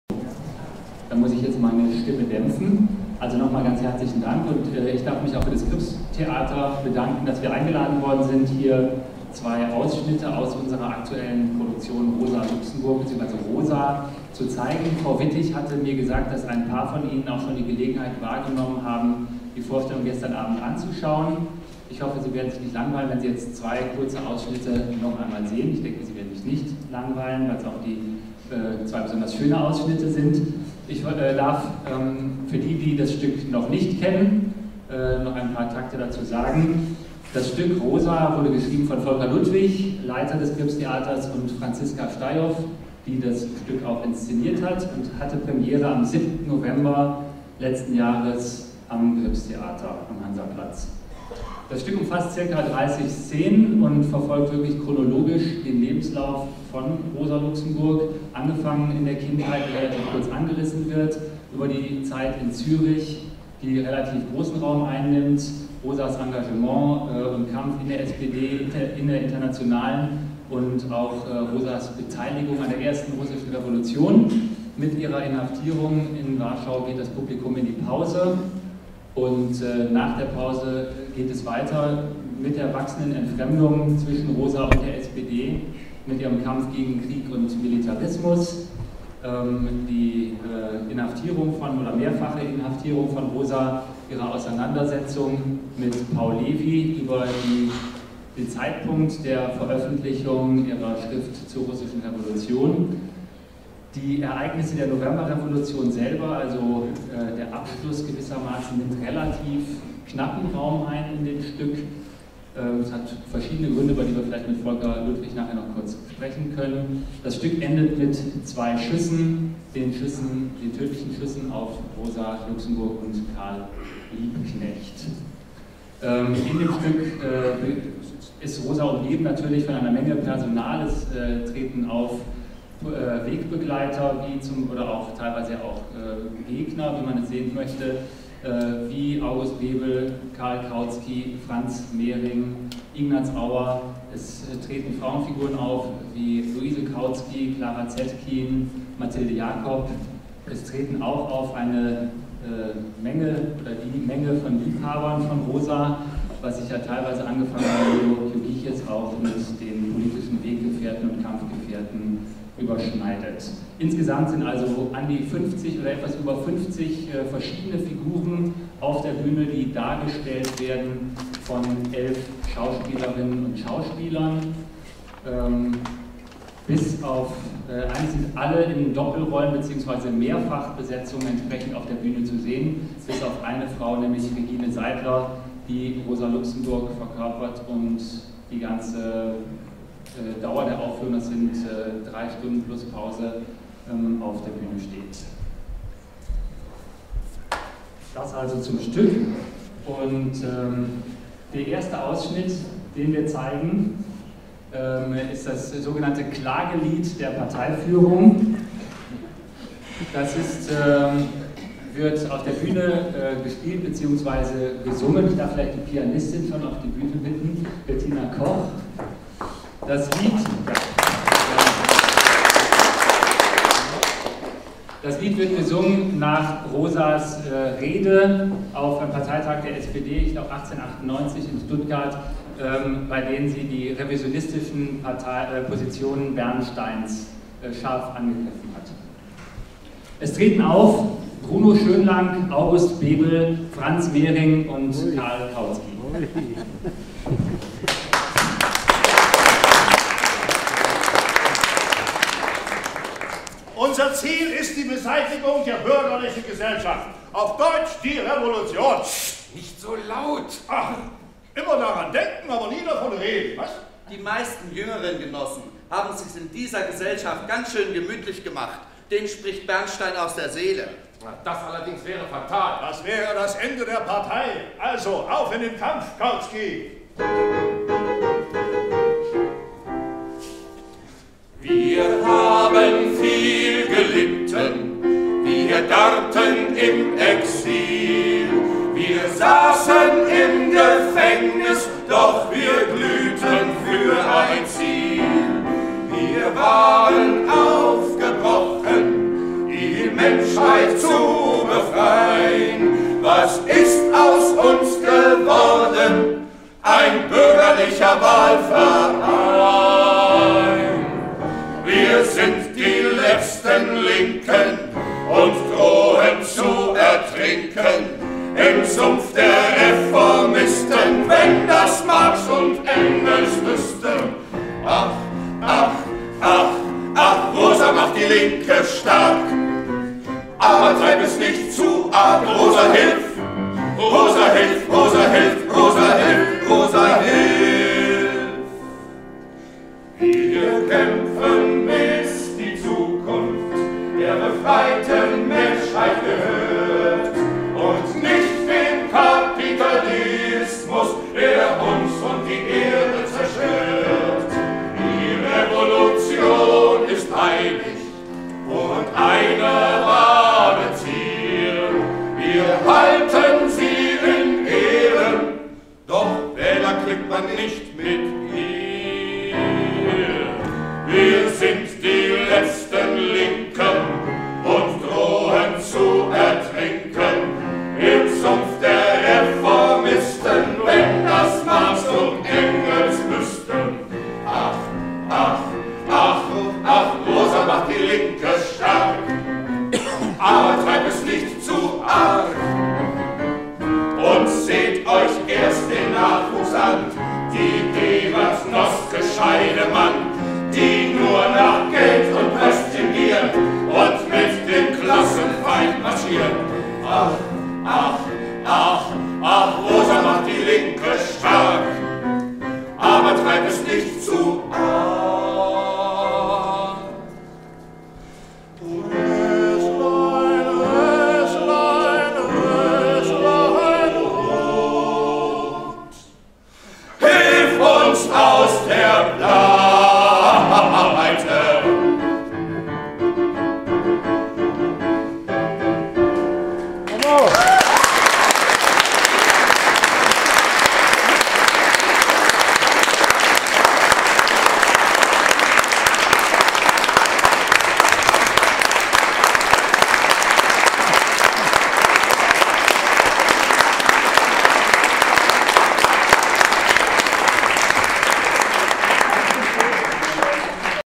»Rosa« Gripstheater Berlin - Szene 1
Schauspieler des Gripstheater-Ensembles spielen Szenen aus ihrem aktuellen Stück über Rosa Luxemburg. Die Szene: »Das Klagelied der Parteiführung« - eine Reaktion auf Rosas Rede auf dem Parteitag in Stuttgart 1889, in der sie die revisionistischen Positionen Bernsteins scharf angegriffen hatte. Es treten auf: Bruno Schönlank, August Bebel, Franz Mehring und Karl Kautsky. Kulturprogramm auf der Internationalen Rosa Luxemburg Konferenz am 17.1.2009 »Mit einem Worte, die Demokratie ist unentbehrlich.« Neue Erkenntnisse über die vor 90 Jahren ermordete Sozialistin und ihre Bedeutung für die Linken der Gegenwart.